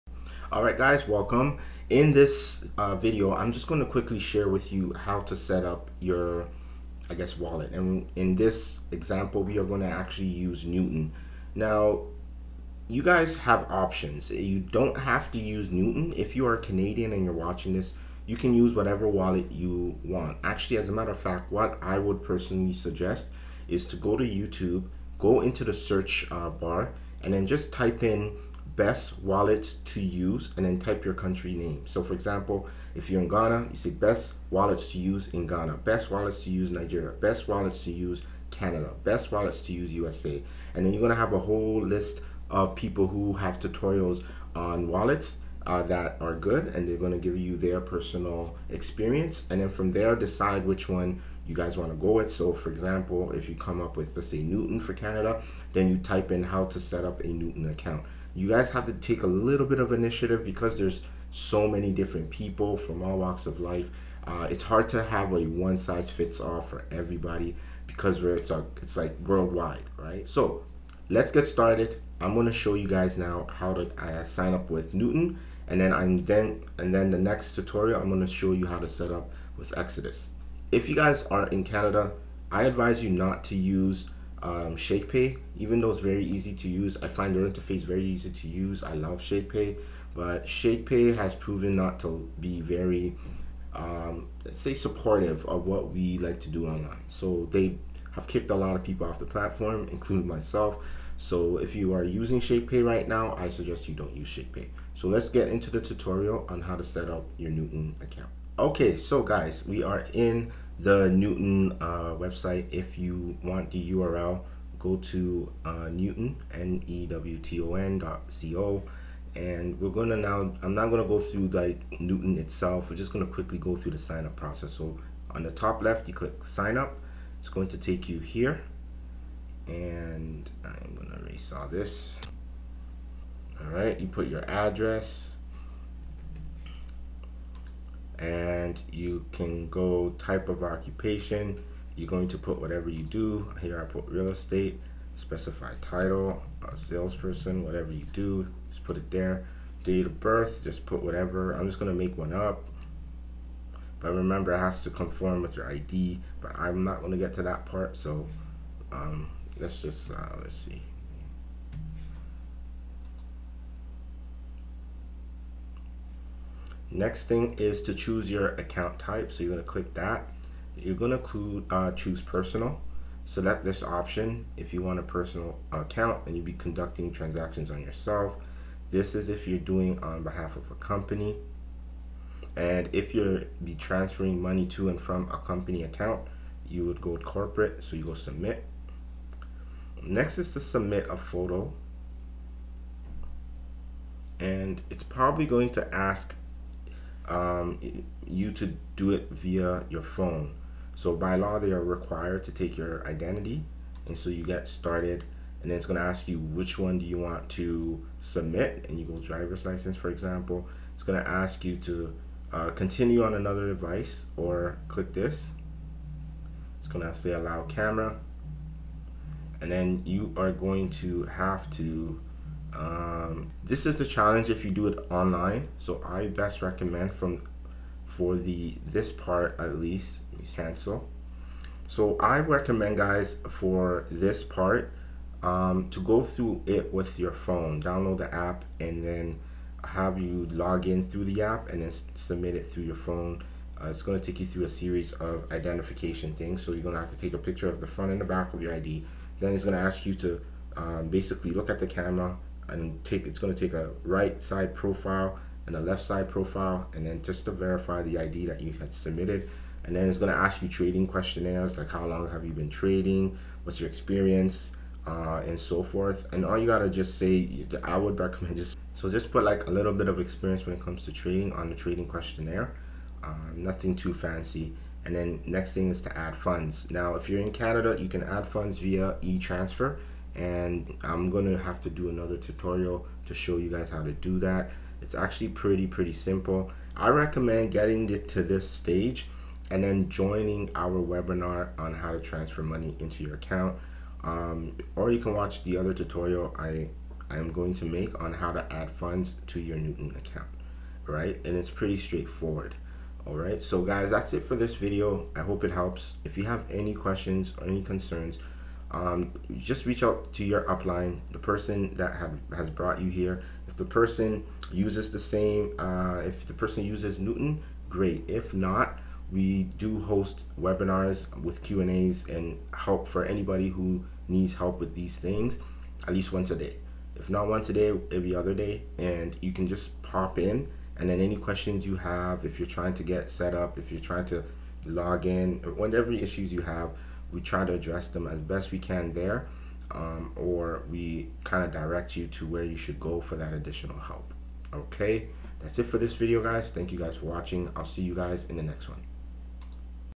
Newton Tutorial: How To Setup Your Newton Account
Newton is a great crypto wallet to use, especially if you're Canadian. Watch the video to learn how you can setup an account.

Join the WMG Community and let's build wealth together